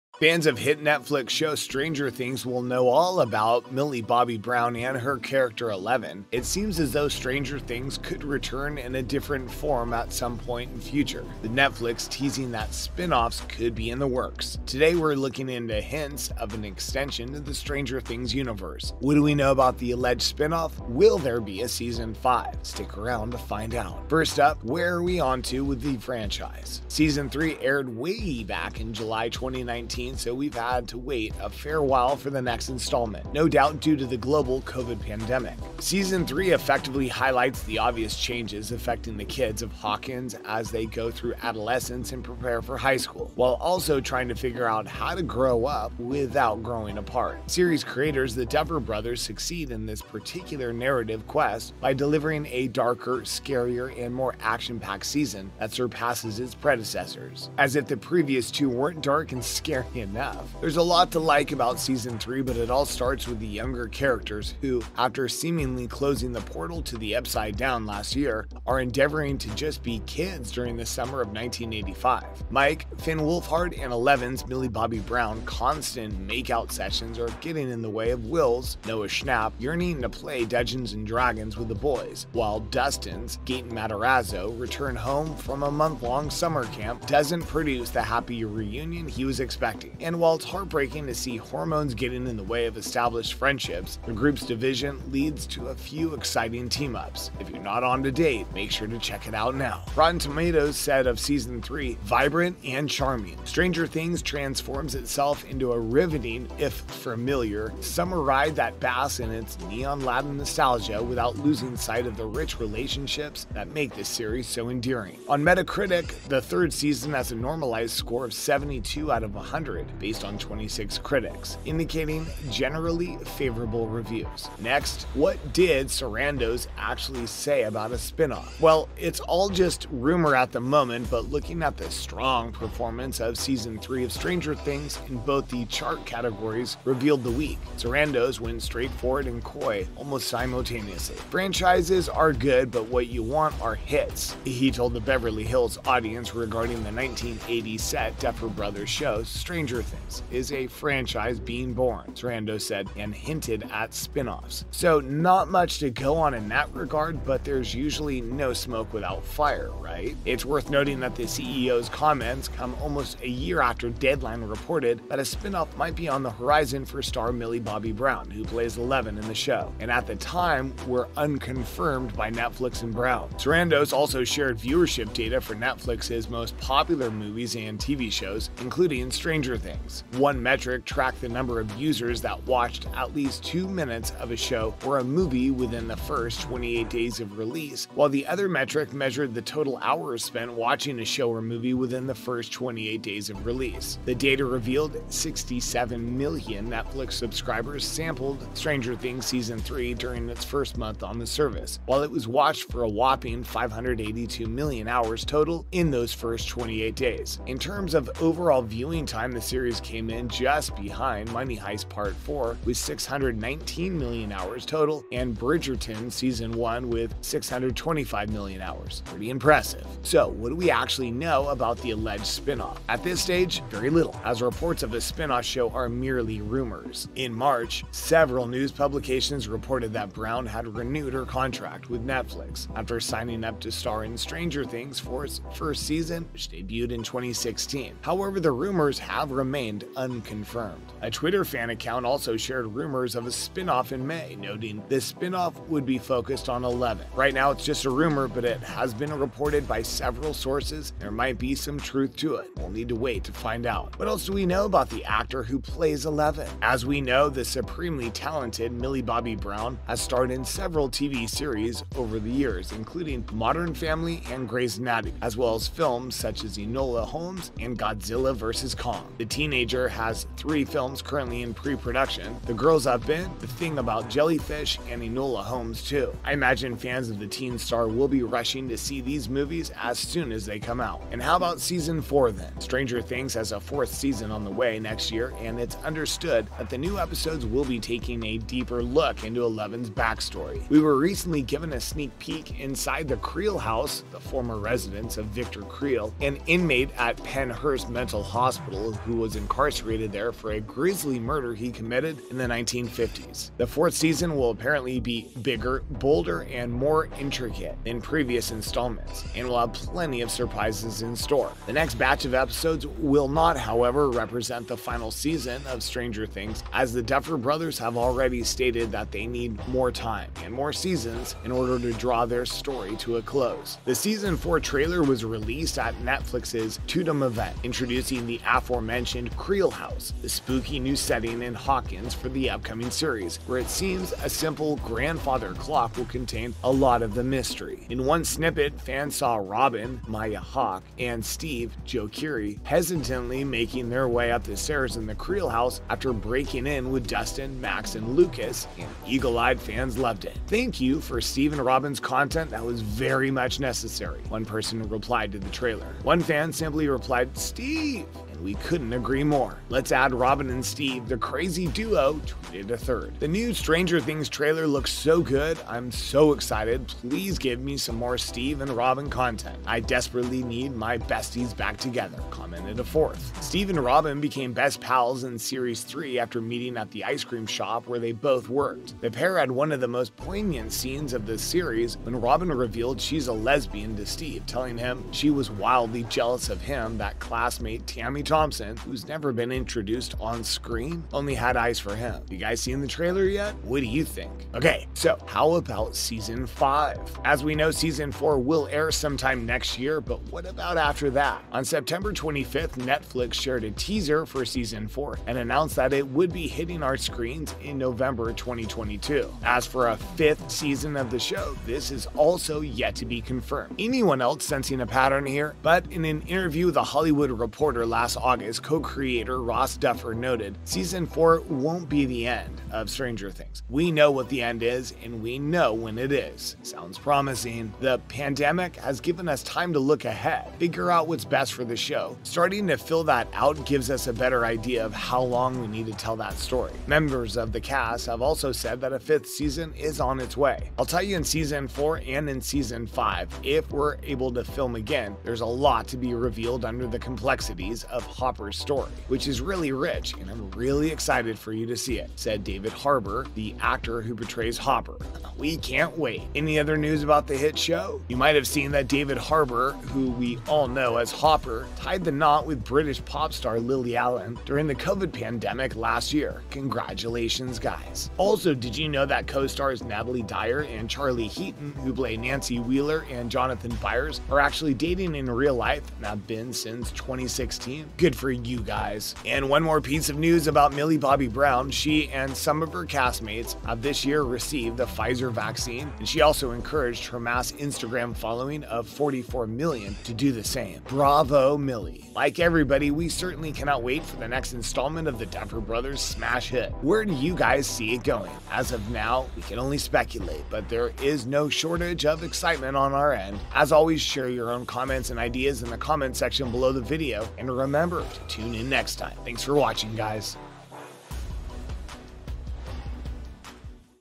Millie Bobby Brown's 'Eleven' Is Getting Her Own Show.. Here's What You Need To Know!
Welcome back to The Showest, today on the channel we are going to talk about  Netflix Exec Teases 'Stranger Things' Spinoff for Millie Bobby Brown's Eleven here's everything you need to know + other related news. Stranger Things is already three *huge* seasons in (with a fourth excitingly on the way), but it looks like Netflix might not be done with the show just yet. While a fifth season is yet to be confirmed, it seems as though Stranger Things could return in a different form, with Netflix teasing that spin-offs could be in the works - including one for Millie Bobby Brown's character Eleven.

Netflix co-CEO Ted Sarandos discussed his plans for the show at the Code Conference in Beverly Hills earlier this week. Speaking to the audience, Sarandos said, "Franchises are good, but what you want are hits," according to Deadline. He then reportedly called Stranger Things "a franchise being born," before hinting at "spinoffs". Deadline also reports that there has been 'talk' that Millie Bobby Brown could be 'taking the lead' in an extension of the Stranger Things universe under her own Netflix deal. So, watch out for a potential Eleven solo special, then. Though Netflix is yet to confirm season five for Stranger Things, they have said there's still more to come from the show. Speaking to The Hollywood Reporter showrunners and executive producers Matt and Ross Duffer previously revealed, "Season four won't be the end. We know what the end is, and we know when it is. [The pandemic] has given us time to look ahead, figure out what is best for the show. Starting to fill that out gave us a better idea of how long we need to tell that story." Find out more about the 'Stranger Things' Spinoff for Millie Bobby Brown's Eleven in this video.